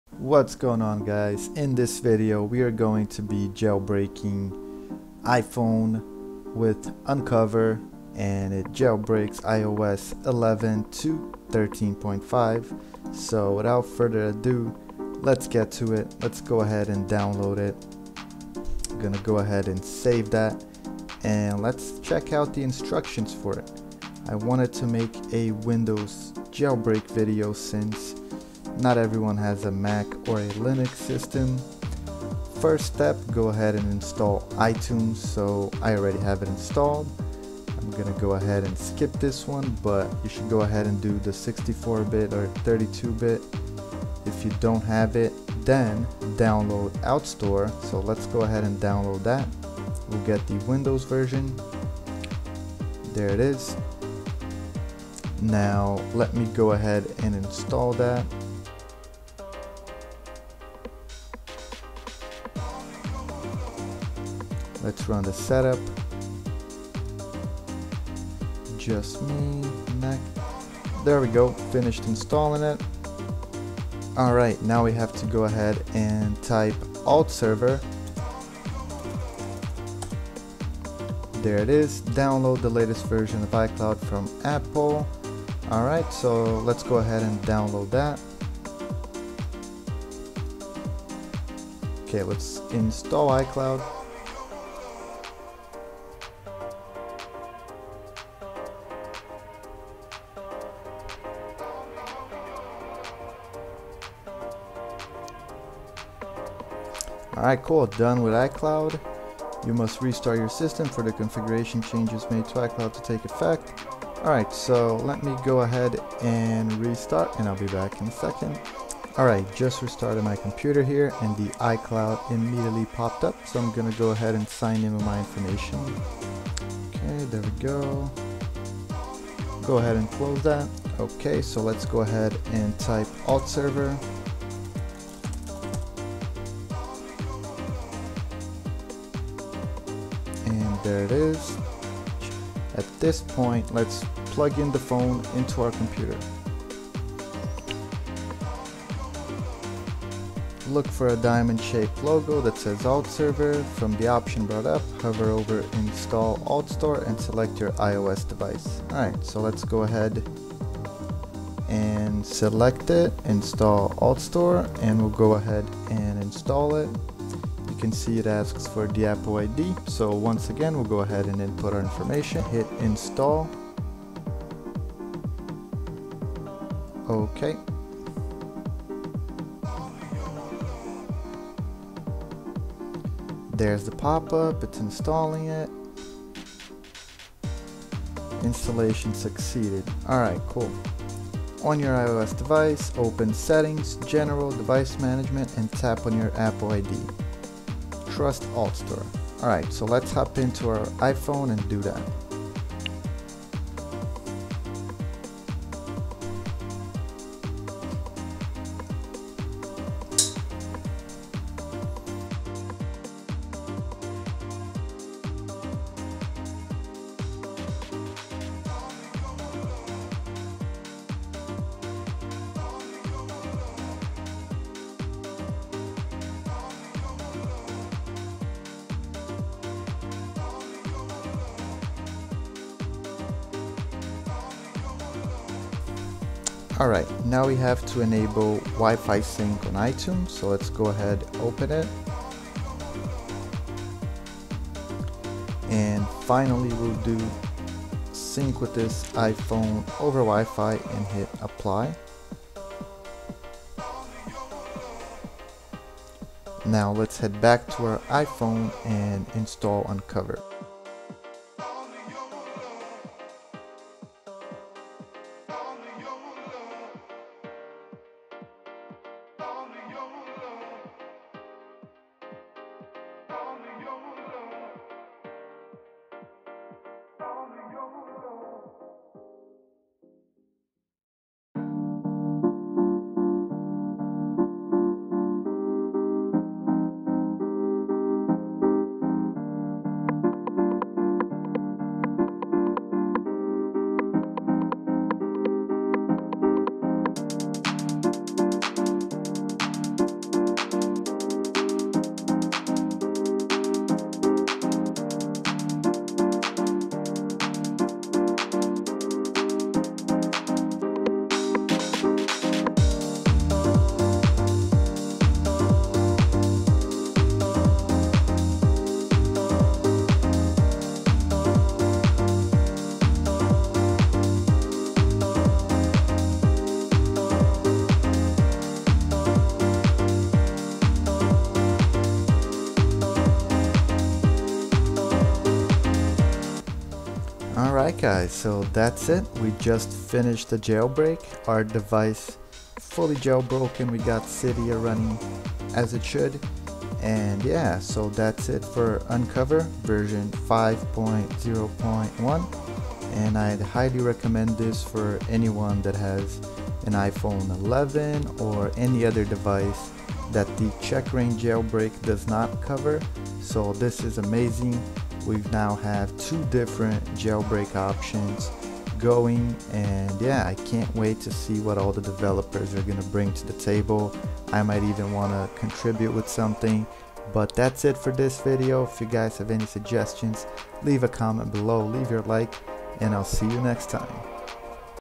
iOS 11 - 13.5 Jailbreak on Windows ( unc0ver )
In this video we'll be jailbreaking an iOS 13.5 iPhone using unc0ver.
I will be doing it under Windows although mac and Linux is also supported.

This is really cool since it allows to jailbreak devices that are unsupported by checkra1n, a previous jailbreak that came out late last year.